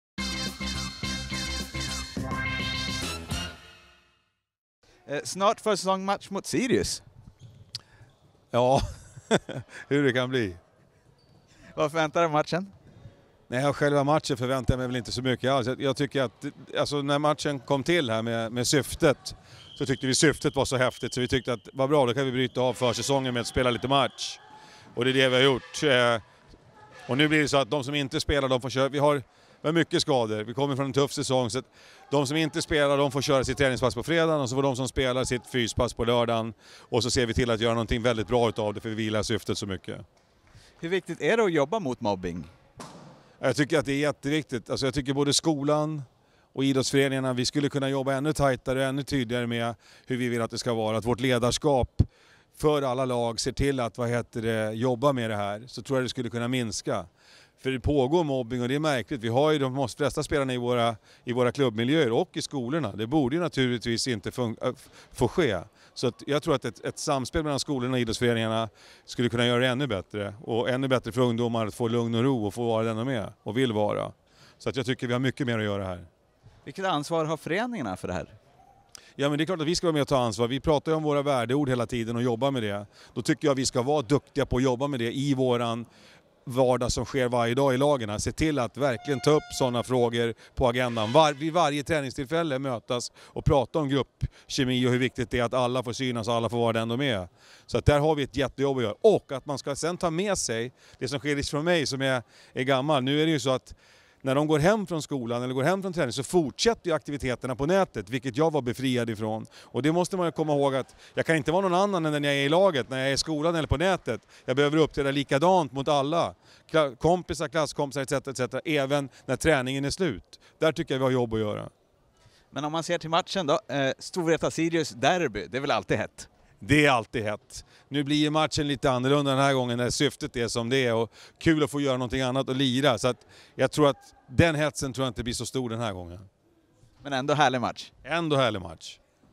Stefan Forsman: Det är jätteviktigt! 20/5-2017
Idag är det dags för välgörenhetsmatch mellan Storvreta IBK och IK Sirius IBK i Olandshallen i kampen mot mobbning. Storvreta IBK:s tränare Stefan Forsman tar chansen att reflektera över matchen och syftet i senaste Storvreta News.